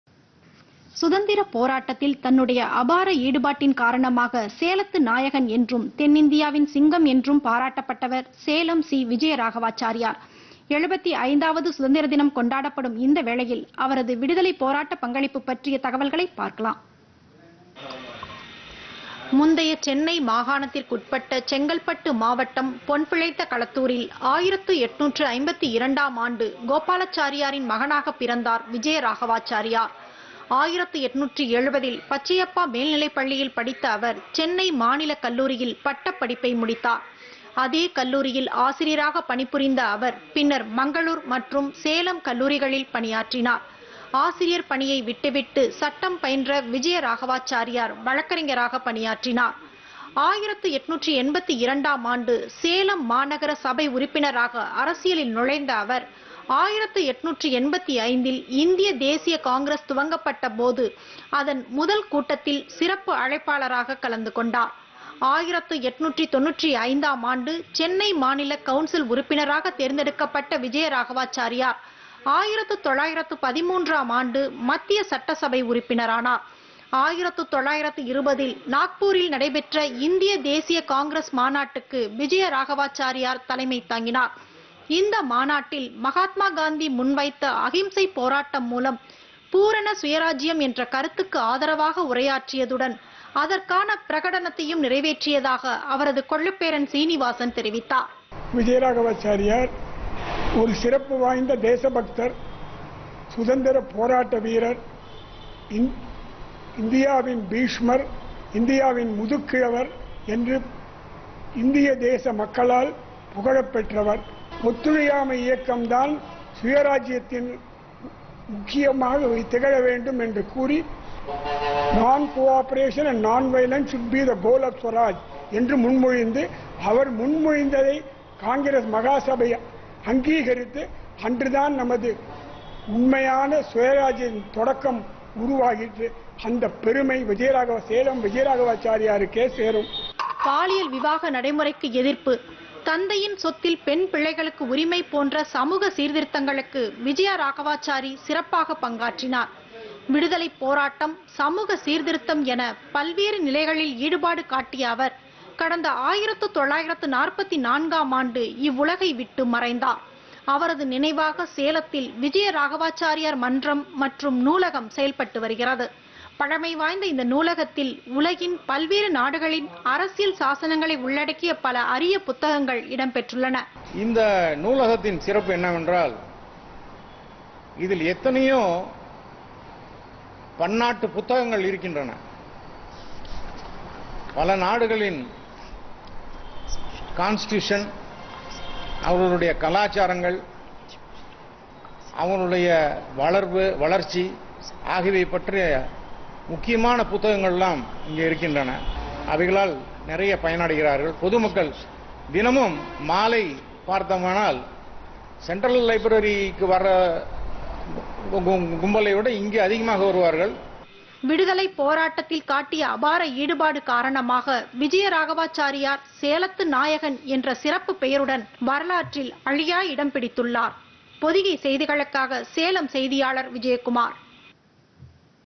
PodhigaiTamilNews பொதிகைசெய்திகள் AzadiKaAmritMahotsav
சுதந்திர போராட்டத்தில் தன்னு‍டைய அபார ஈடுபாட்டின் காரணமாக சேலத்து நாயகன் என்றும், தென்னிந்தியாவின் சிங்கம் என்றும் பாராட்டப்பட்டவர்   சேலம்  சி விஜயராகவாச்சாரியார்.
  75 வது சுதந்திர தின கொண்டாடப்படும் இந்த வேளையில் அவரது விடுதலை போராட்ட பங்களிப்பு பற்றிய தகவல்களை பார்க்கலாம்.